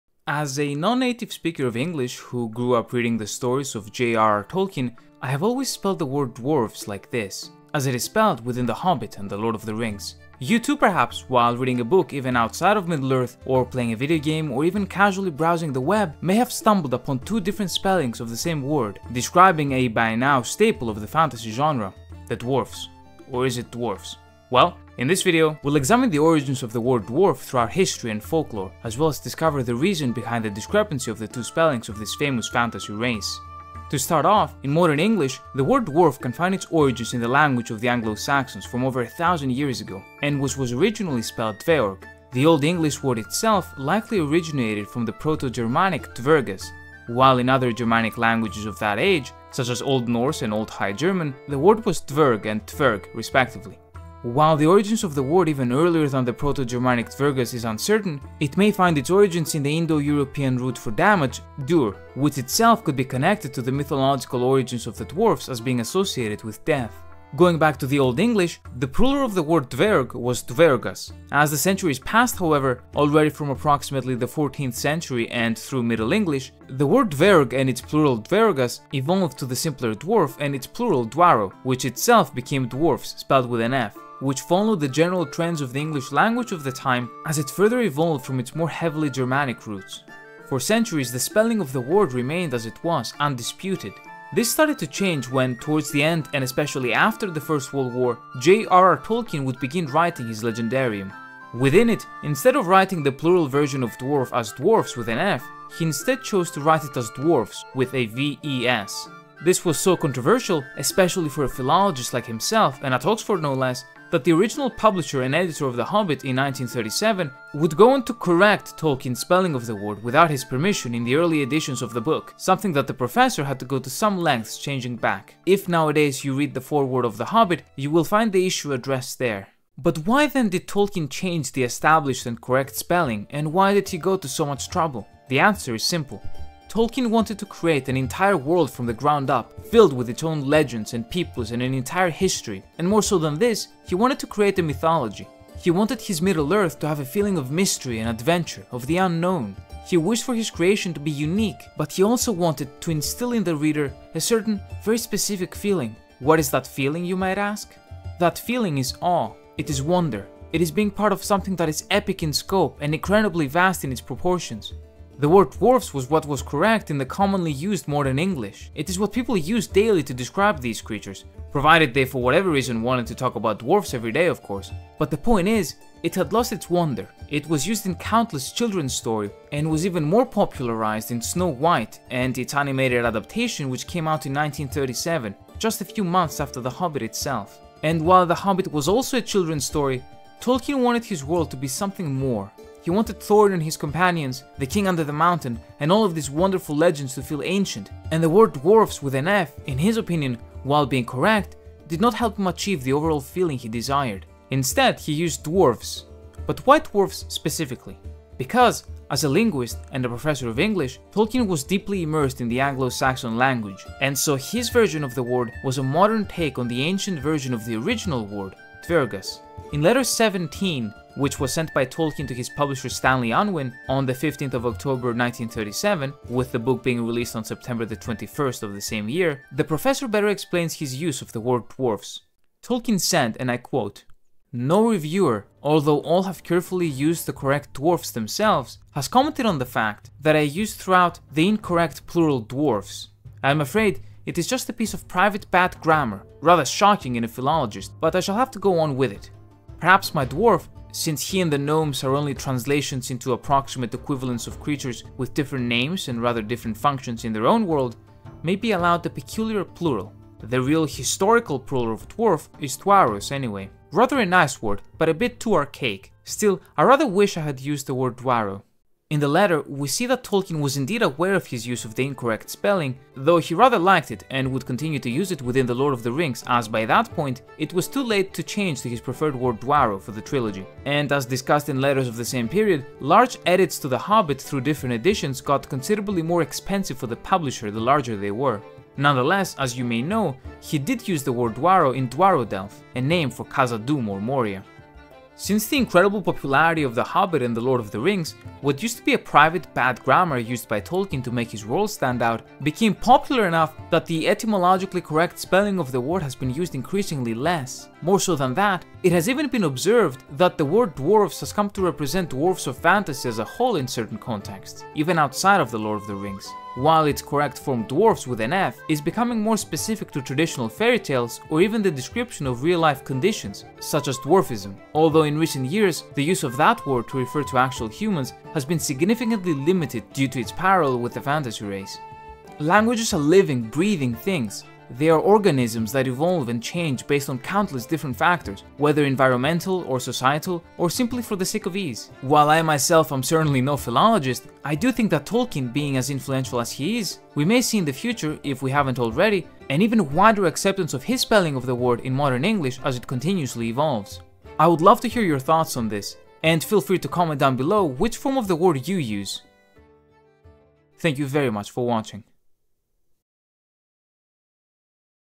Is it Dwarfs or Dwarves?
In this video we explore the history of the word dwarf and the different spellings of its plural form  dwarfs and dwarves throughout history, in fantasy, as well as its origins in the Lord of the Rings.

I would like to make an addition, to further clarify something that may not be as clear as it should be within the video.

In Appendix F of the Lord of the Rings, Tolkien talks about what I mentioned in the video, which is the reason behind his use of the word dwarves instead of dwarfs. The explanation he provides is that the latter has become associated with children's tales, and he wished something more serious for his own dwarves.

As mentioned in letter 17, he also provides an in-lore explanation, with the words he chooses simply being "translations" used to describe what are actually different creatures, leading to what he calls a peculiar plural.

What I would like to clarify further, is that in the very very beginning, as showcased by other letters and interviews, his use of dwarves instead of dwarfs was indeed a grammatical error. Once he realized it, he decided he liked it anyway and continued to use and justify it in the way I describe in the video!

Any feedback is always welcome, I hope you enjoy!!